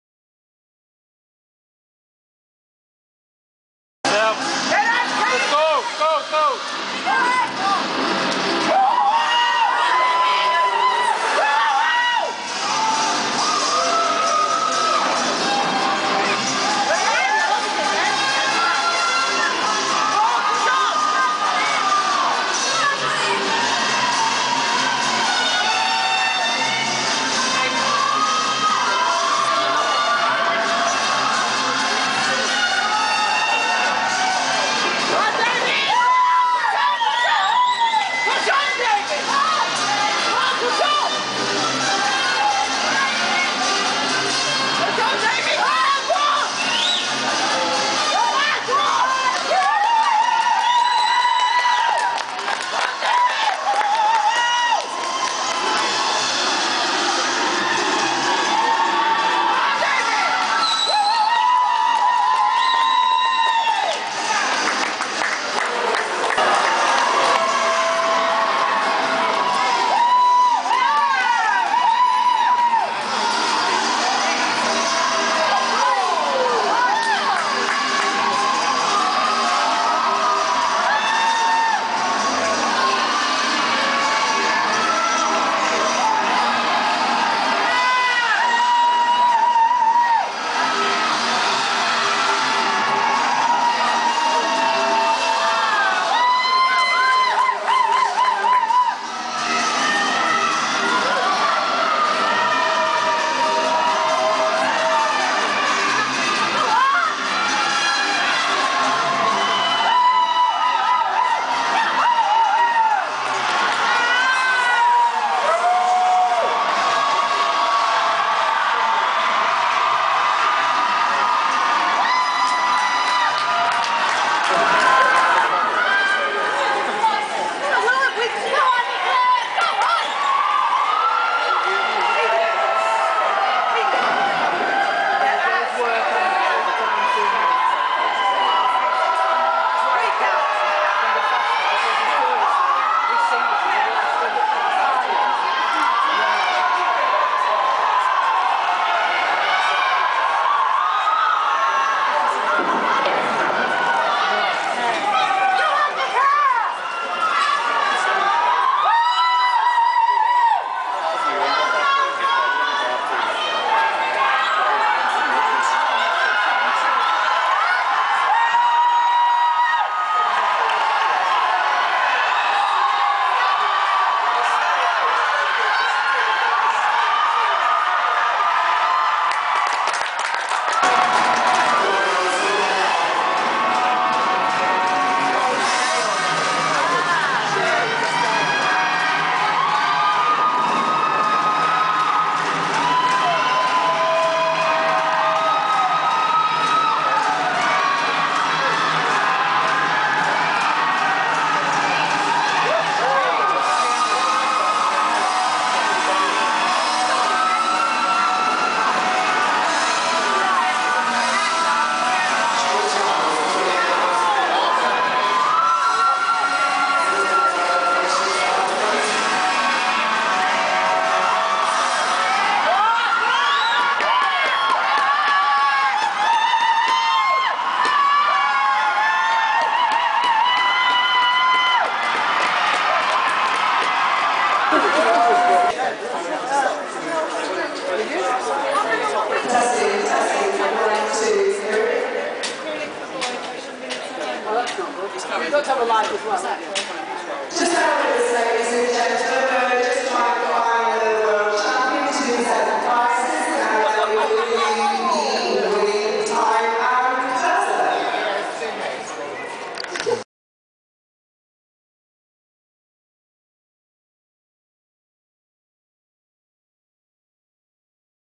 Carriage Driving Speed Trials Round 3 Final
U.K Indoor Carriage Driving Speed Trials Championships Held At Keysoe Round Three Final